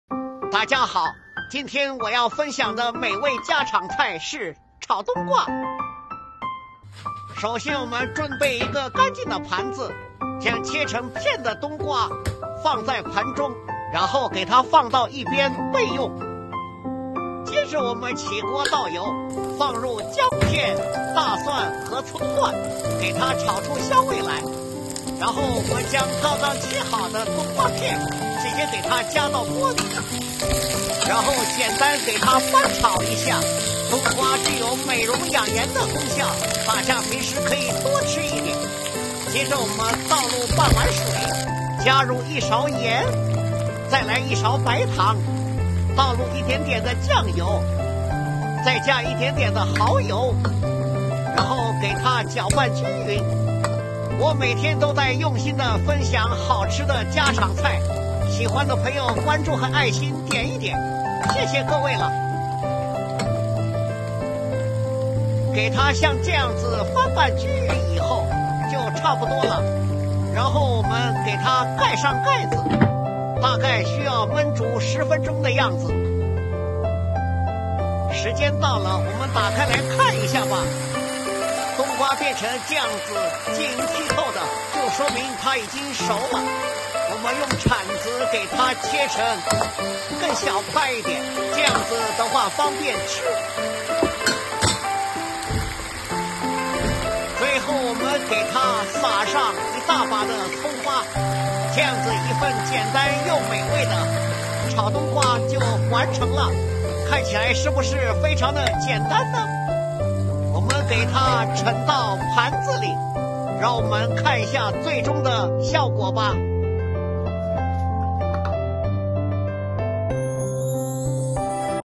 家常炒冬瓜，味道确实不错，补水解暑夏天的下饭菜#delicious
冬瓜包括果肉、瓤和籽，含有丰富的的蛋白质、碳水化合物、维生素以及矿质元素等营养成分。
冬瓜味甘、性寒，有消热、利水、消肿的功效。